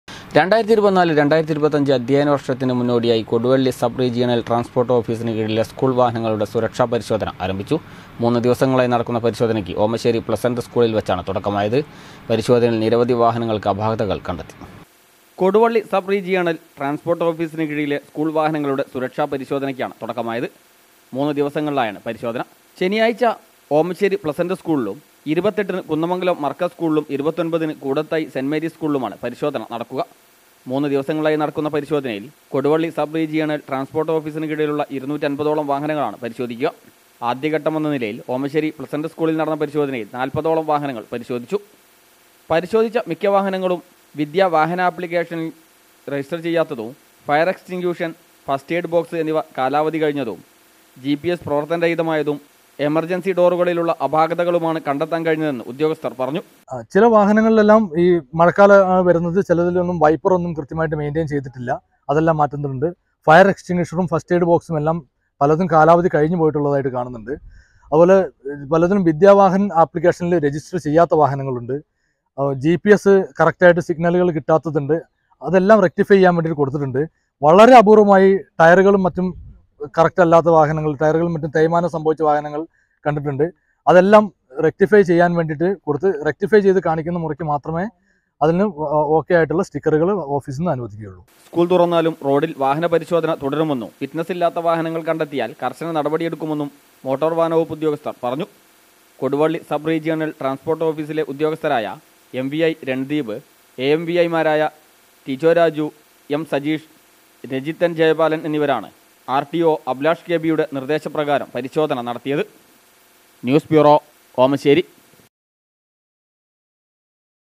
കൊടുവള്ളി സബ് റീജണൽ ട്രാൻസ്‌പോർട്ട് ഓഫീസിന് കീഴിലെ സ്കൂൾ വാഹനങ്ങളുടെ സുരക്ഷാ പരിശോധന ആരംഭിച്ചു.
2024-25 അധ്യയന വർഷത്തിന്റെ മുന്നോടിയായി കൊടുവള്ളി സബ് റീജണൽ ട്രാൻസ്‌പോർട്ട് ഓഫീസിന് കീഴിലെ സ്കൂൾ വാഹനങ്ങളുടെ സുരക്ഷാ പരിശോധന ആരംഭിച്ചു.